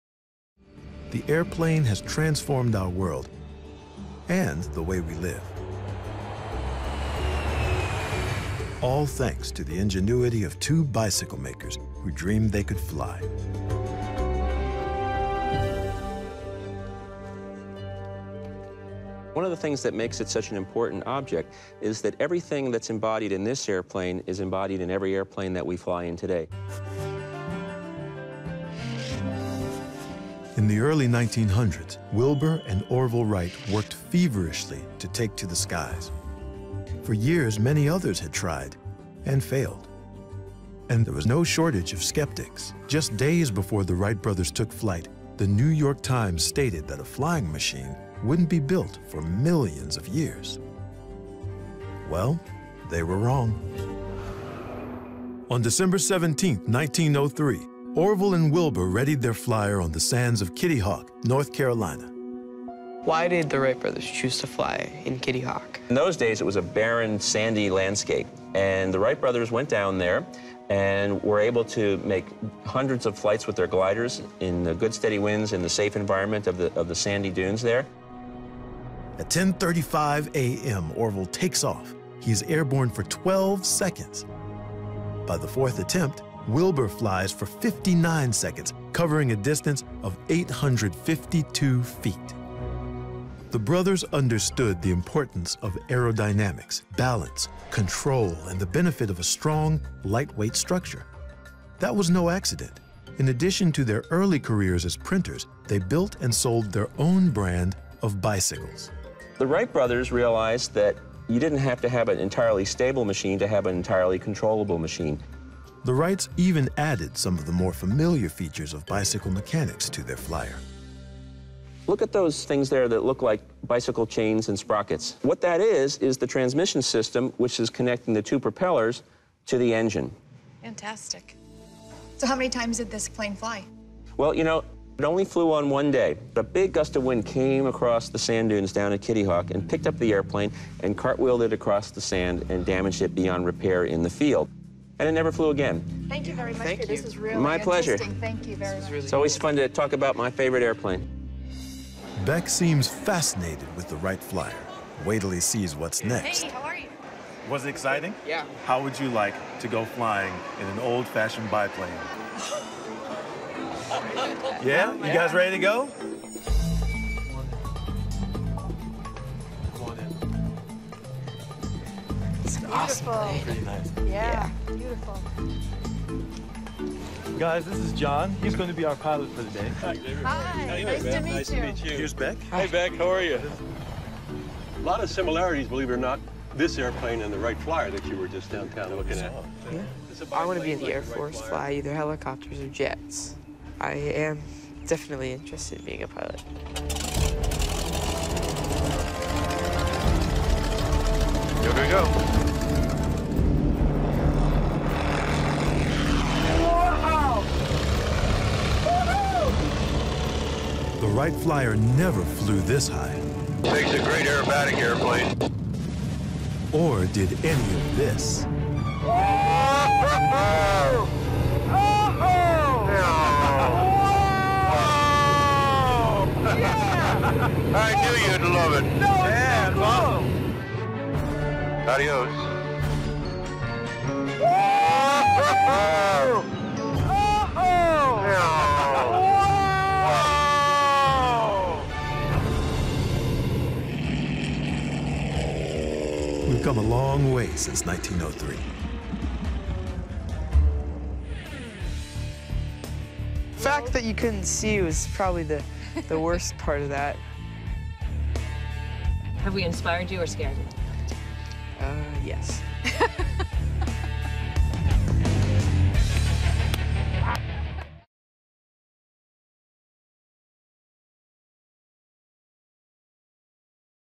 The Most Priceless Aviation Artifact in History
The airplane has transformed our world and it's all thanks to the ingenuity of two bicycle makers, Wilbur and Orville Wright.